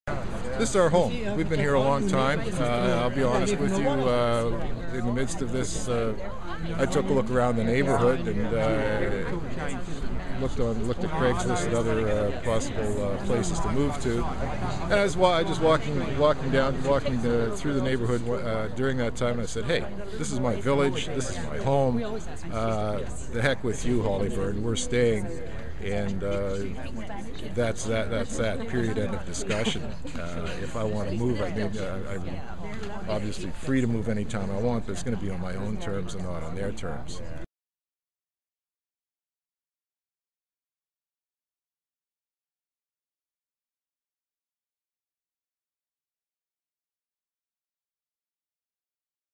Emerald Terrace residents win against alleged 'renovictions'
Residents of a West End apartment building have won a fourth decision against landlord Hollyburn Properties. The residents won the backing of an arbitrator at the Residential Tenancy Branch, who suggested Hollyburn had "misinformed" tenants about proposed renovations.